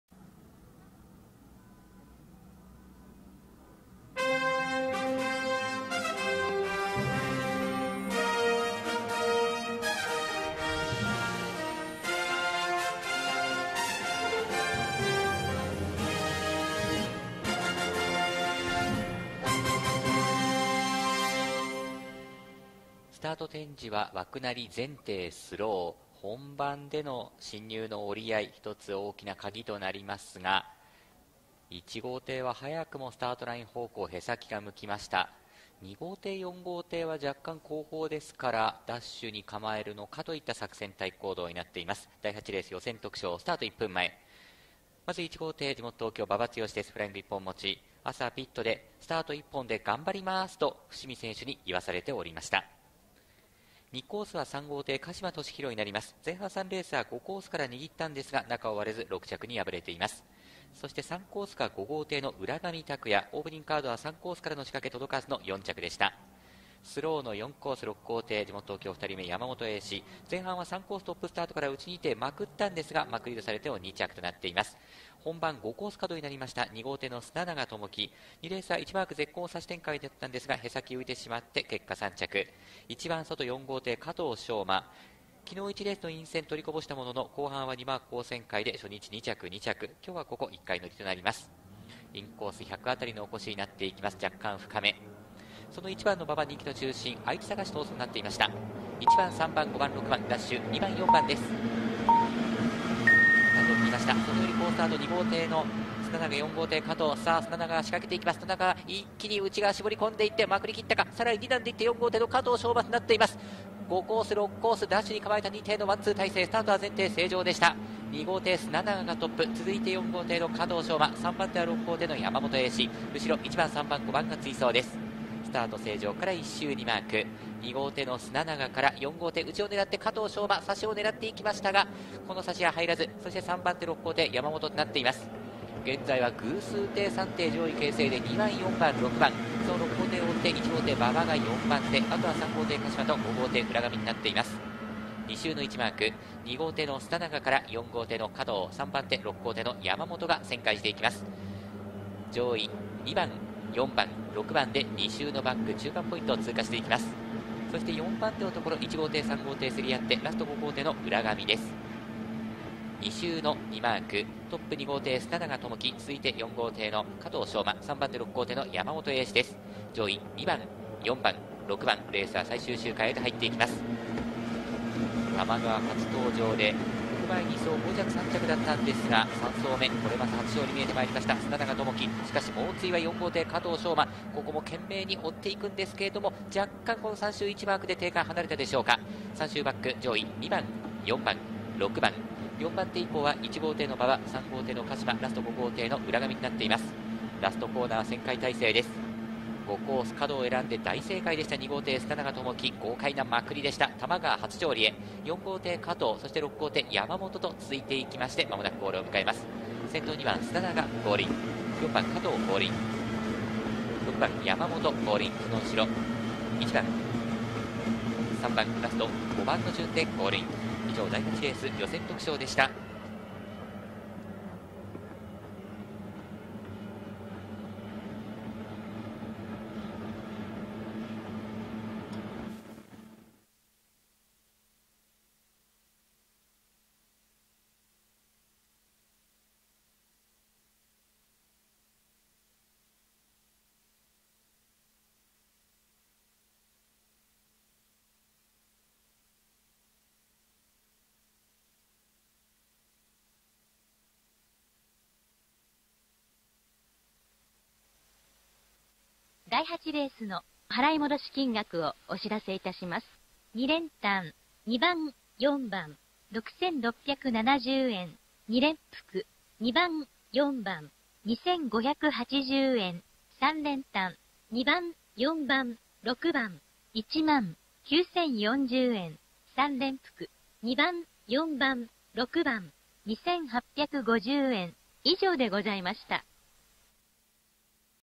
超新星☆5112 砂長知輝が豪快なカドまくりで多摩川初勝利！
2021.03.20 ボートレース多摩川
第16回澤乃井カップ2日目8R
①馬場剛 ②砂長知輝 ③鹿島敏弘 ④加藤翔馬 ⑤浦上拓也 ⑥山本英志
スタート展示は枠なりでしたが本番レースでは意表を突くカド5コース進入
コンマ12のスタートからまくり一撃快勝！
多摩川初勝利で通算11勝目。